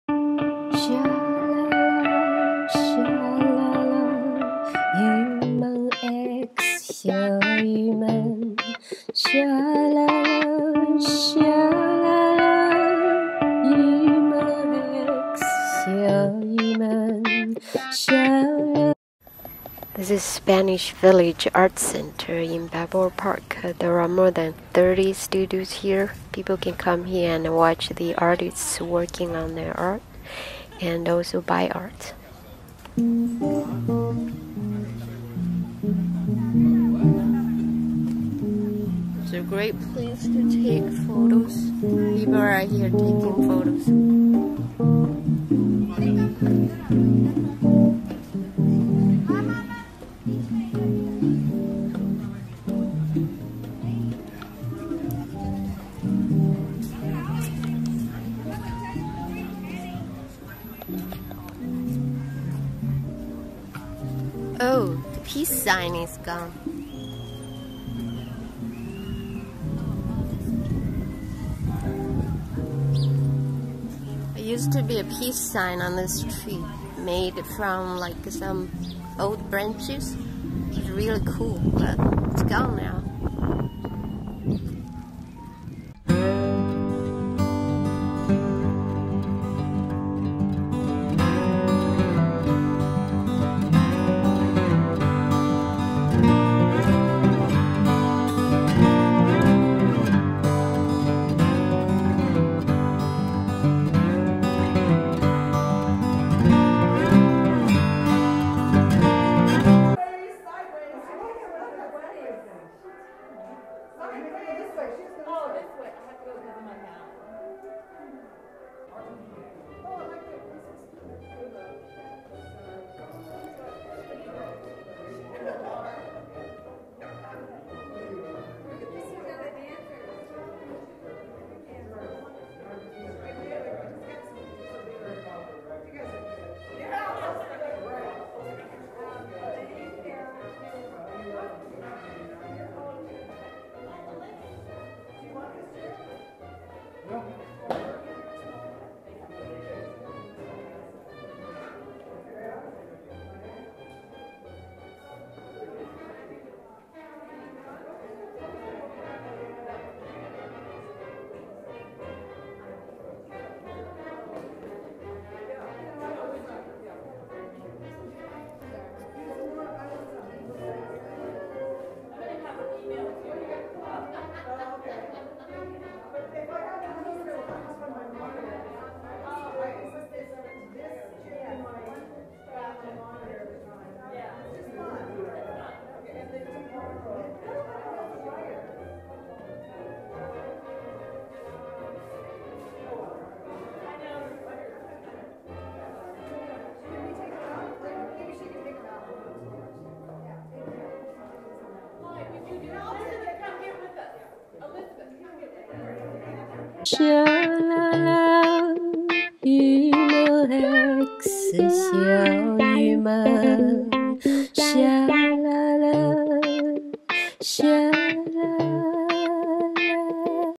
Miscellaneous Videos & Photos Of Spanish Village Art Center Balboa Park
This video includes a short video of the SPVA patio, a display of some artists dressed in the 1930s' clothes for the historical celebration held a few years ago, and a short video of the wonderful performance of two swing dancers doing Charleston.